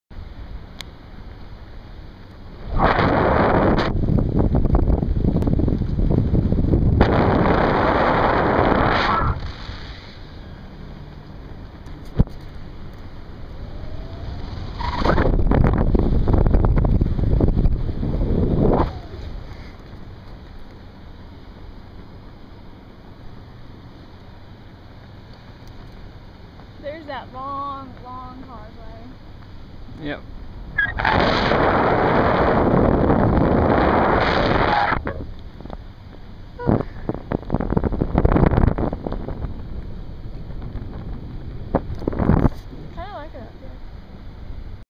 Para-sailing up in the air
Once we got up in the air I took this video of just floating around